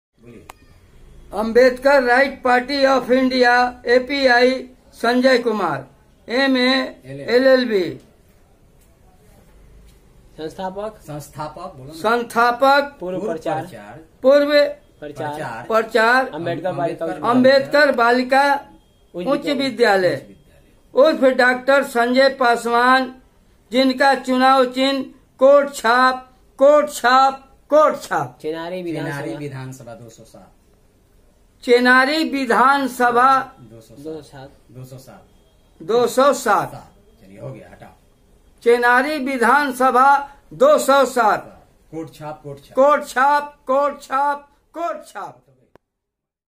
65 साल की उम्र में भी इतना अच्छा एनाउंसर
अंबेडकराइट पार्टी ऑफ इंडिया (API) संजय कुमार उर्फ डॉ संजय पासवान चेनारी विधानसभा 207 शिक्षक उम्मीदवार चुनाव चिन्ह कोट छाप।।।।।।।

 यह वीडियो सिर्फ प्रचार के लिए डाला गया है